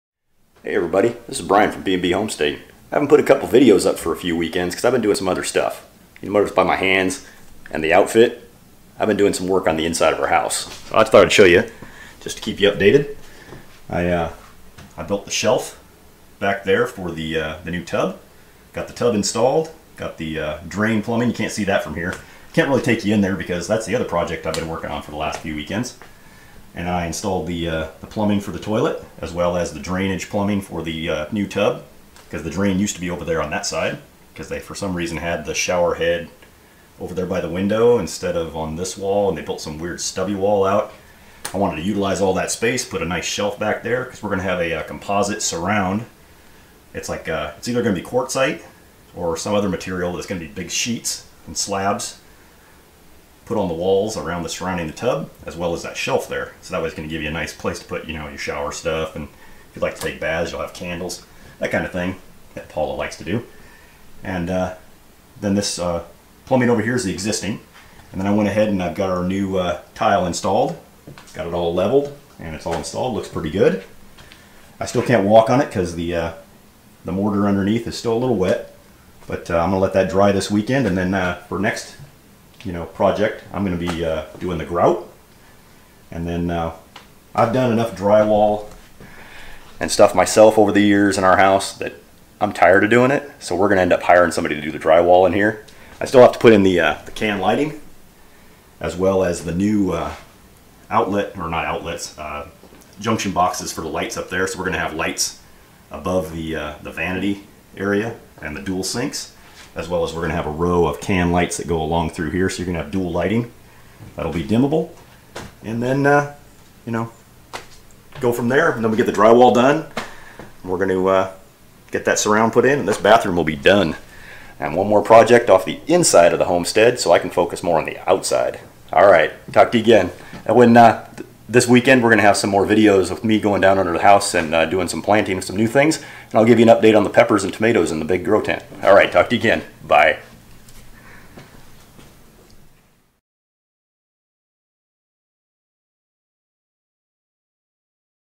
Bathroom remodel on the urban homestead (progress update)
Hi Everyone, I know it's been a couple weeks since I last updated activities on the urban homestead. With the complete lack of outdoor gardening I've started up again on the indoor bathroom remodel project. Here's a quick update for what I've been doing.

Also, I got approval from the boss (Paula) that I can do some planting for the indoor grow tent this weekend, so stay tuned for new videos on that (I'm going to be starting a new large tub of red kale and the peppers and tomatoes are getting pretty good size (will be time to transplant in another 2 weeks to the large black tubs). Enjoy the video update! :-)